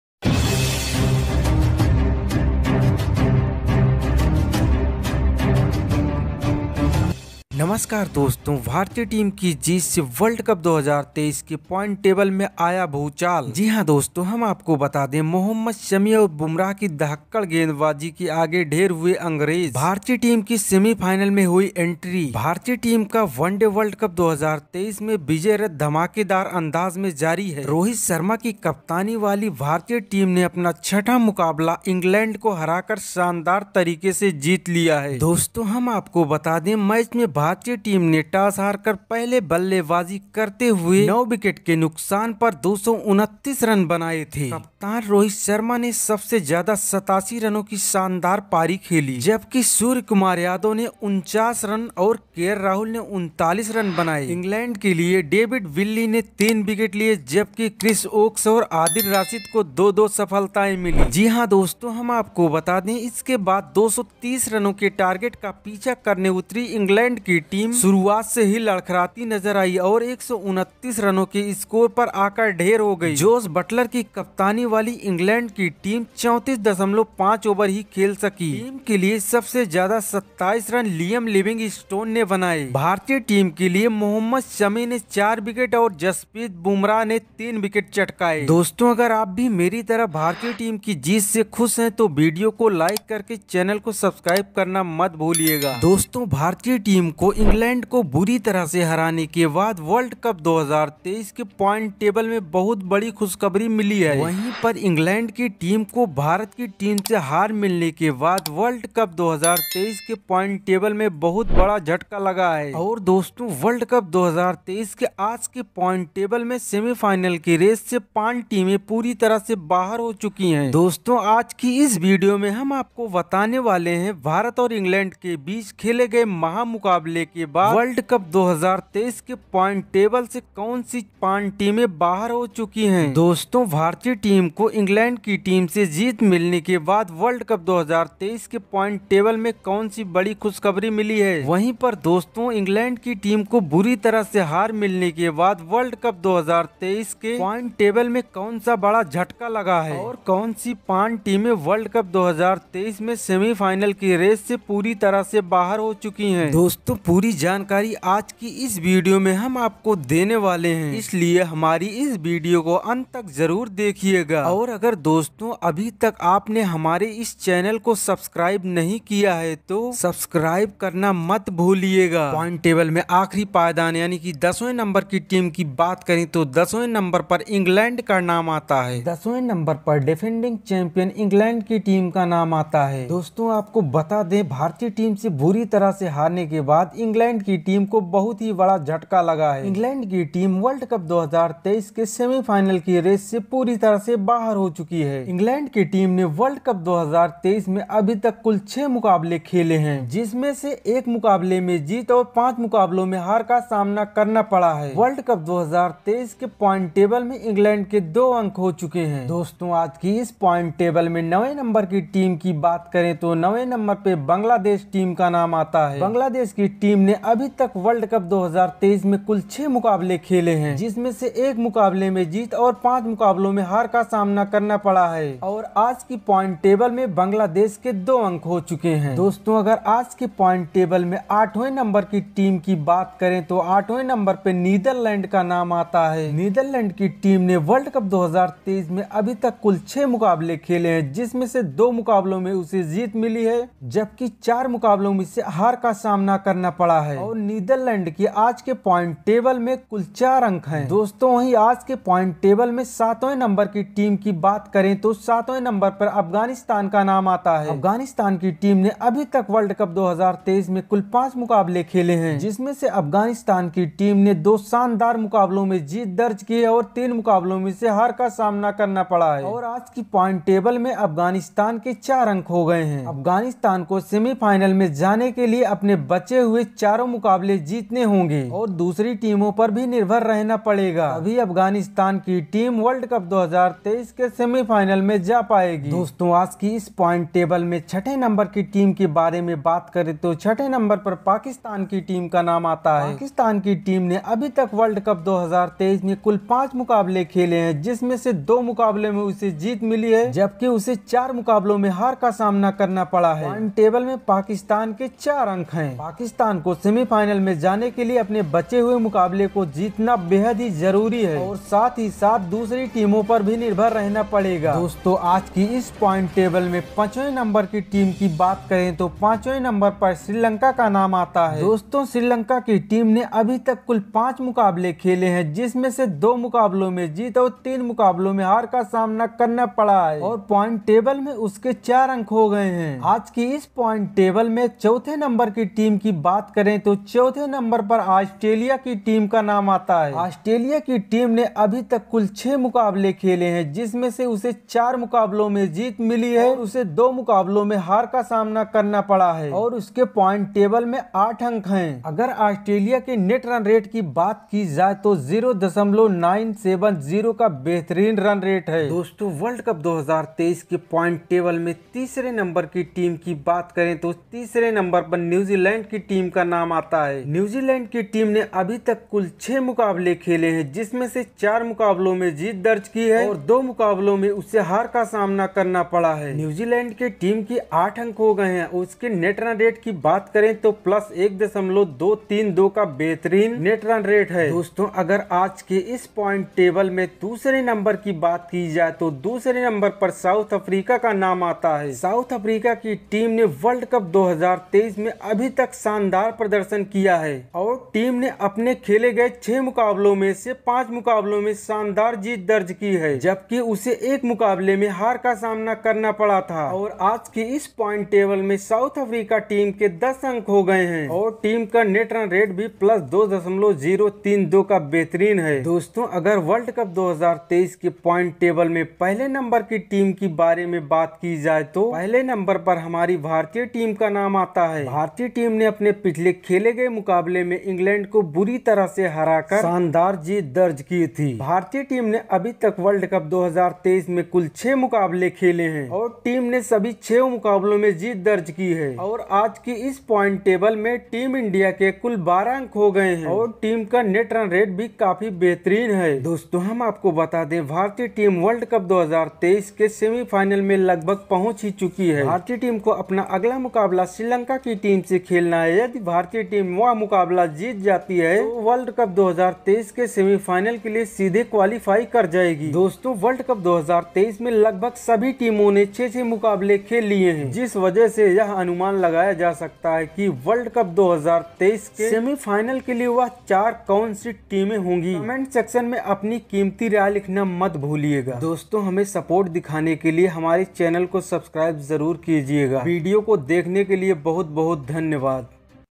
Shocking Upset in World Cup 2023: India Reaches Semi-Finals, ENG and 5 Teams Eliminated
World Cup 2023 Points Table Today, भारत पहुंचा सेमीफाइनल,ENG समेत 5 टीमें WC से बाहर

Credit image by: Bcci&icc